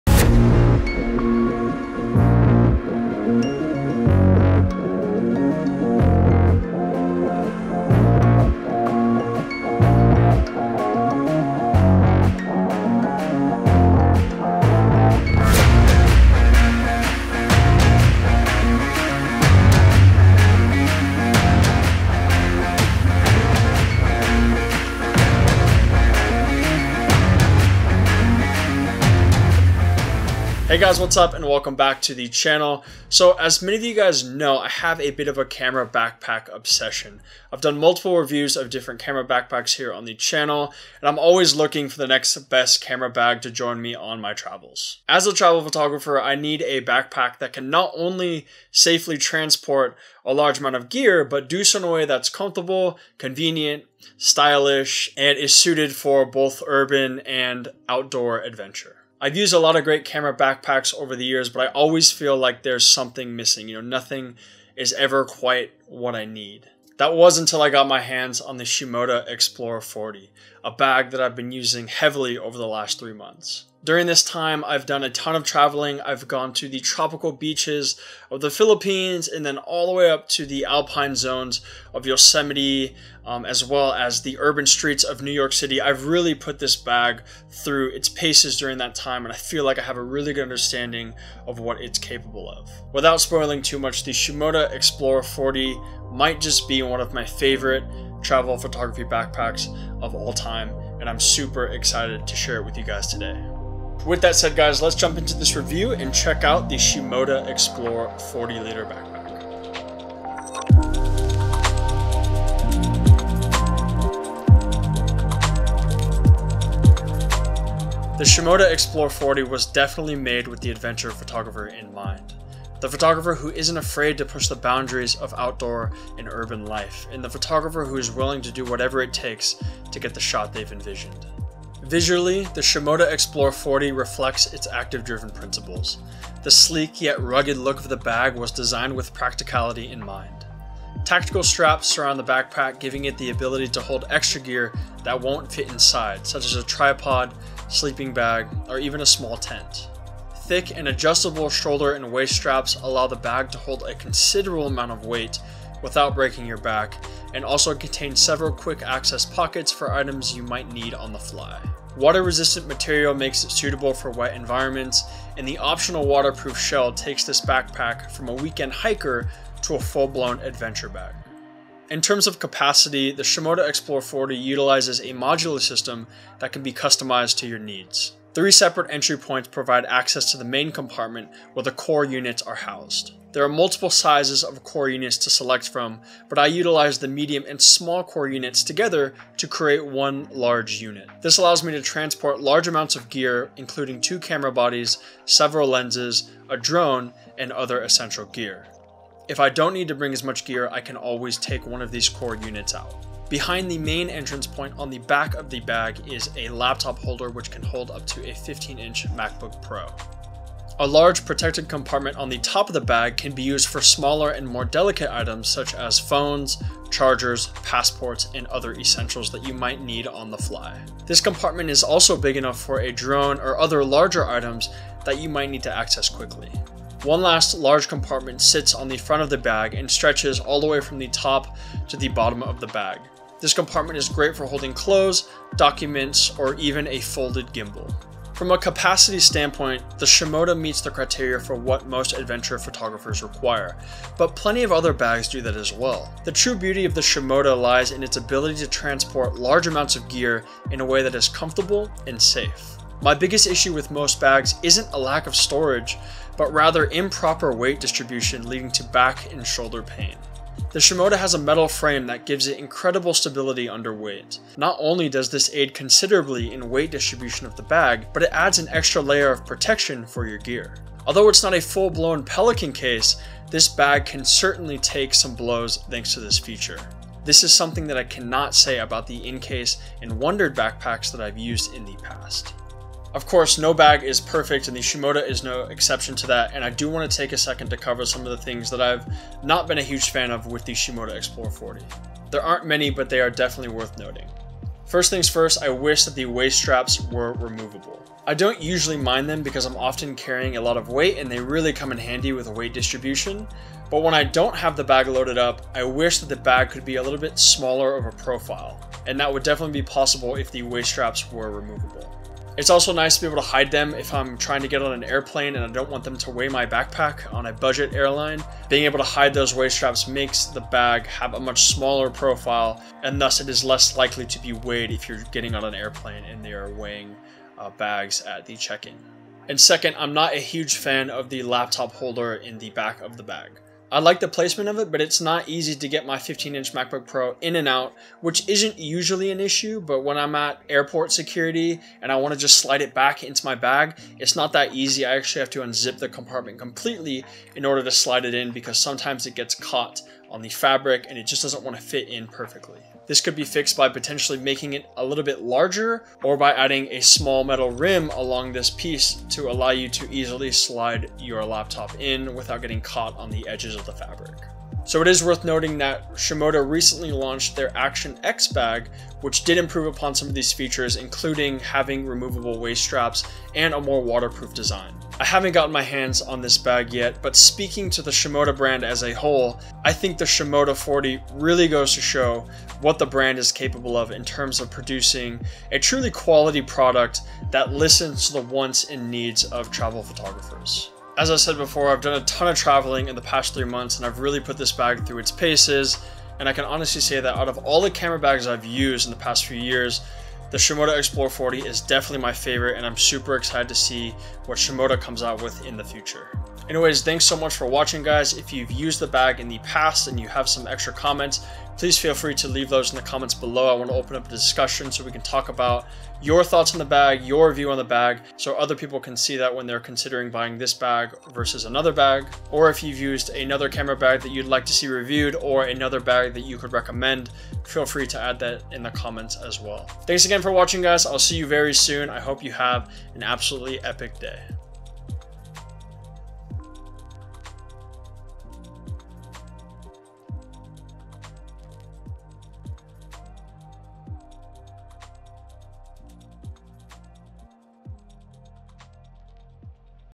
Shimoda Explore 40L Backpack - the best backpack for travel photography?
Is the Shimoda Explore 40L backpack the best travel photography backpack on the market? Shimoda sent over their Explore 40L backpack and I've been using it heavily over the last 3 months. It's a bag I can rely on and I'm excited to finally review it for you guys.

Shimoda didn't pay me for the review, just sent over the bag. Also worth noting that Shimoda recently released their Action X on kickstarter. Hoping to get my hands on this bag to review as well!

Catch you next week!